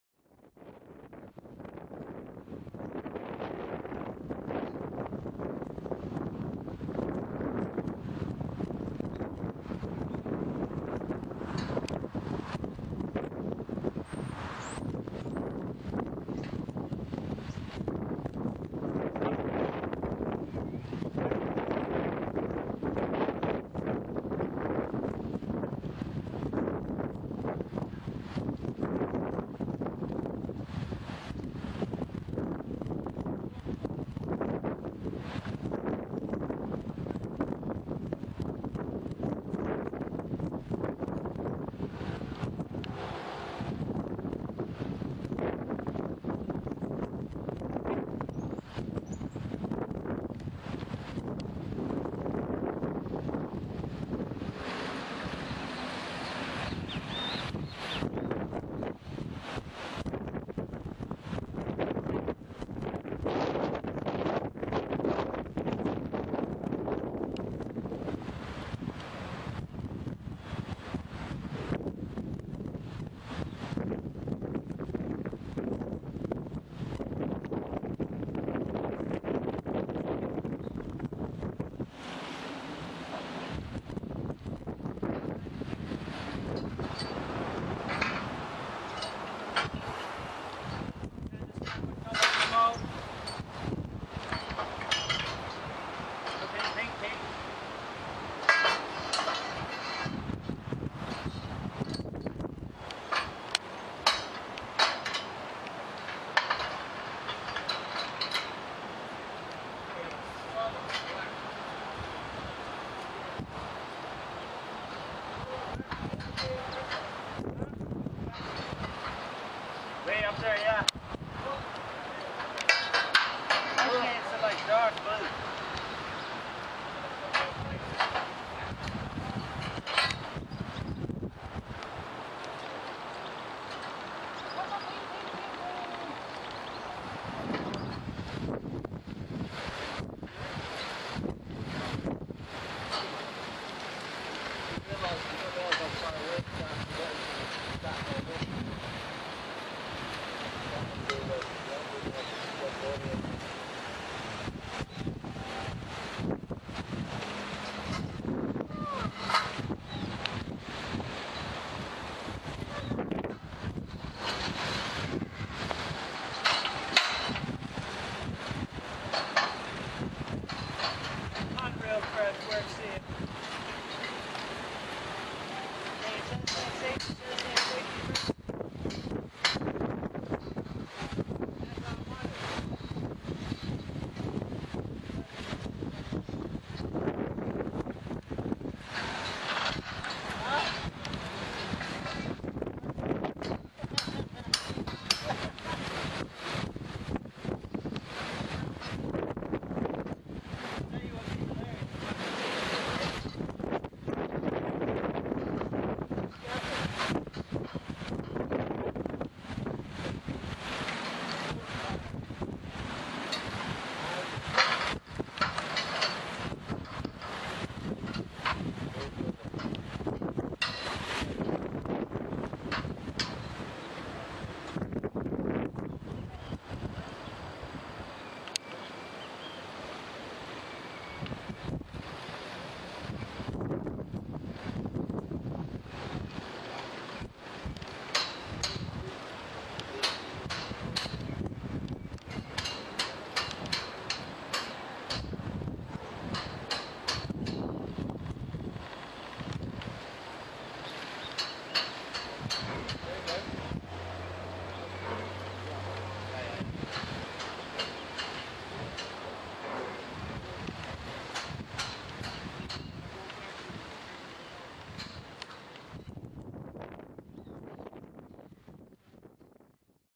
Gibbs Hill Lighthouse - Undergoing MAJOR repairs
Gibbs Hill Lighthouse in the Parish of Southampton undergoing MAJOR repairs.
The last time I remember the Lighthouse being worked on was after Hurricane Fabian in 2003.
There was some major damage done on the island from the hurricane and the lighthouse also sustained damage.
Hopefully I'll be able to catch the work in various stages during the repair process.